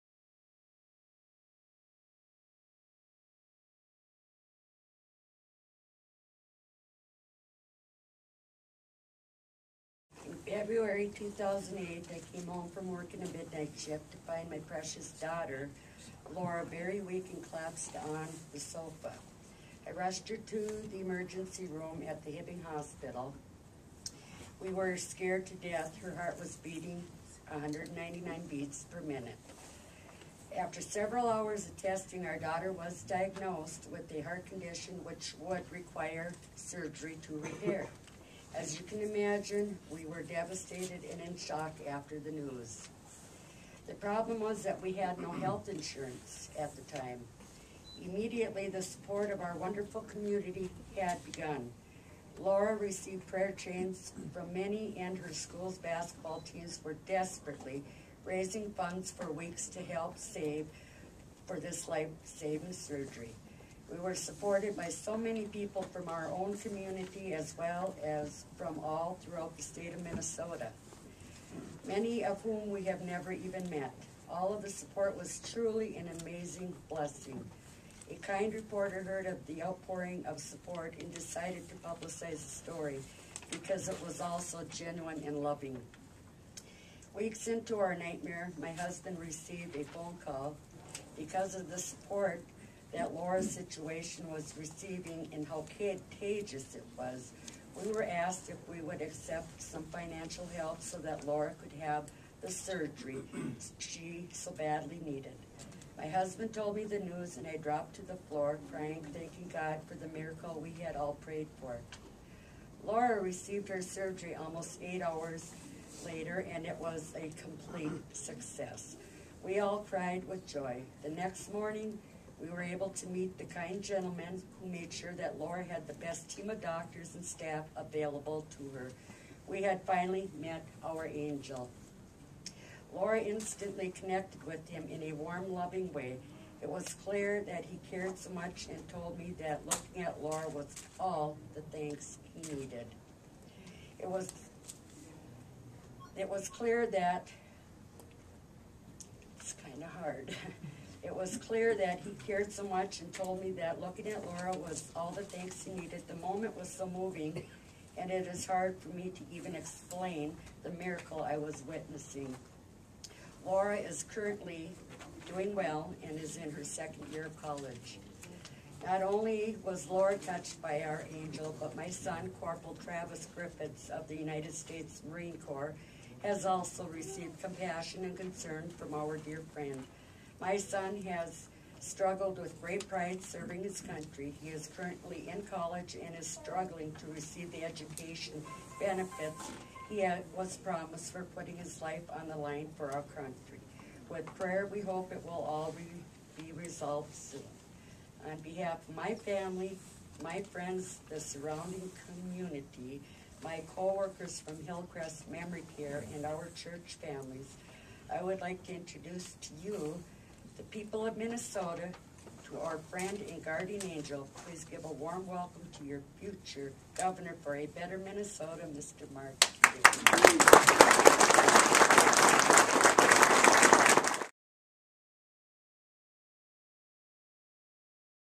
The Sherrie Griffiths Story
Sherrie Griffiths tells how Mark Dayton helped her daughter receive the health care that she needed.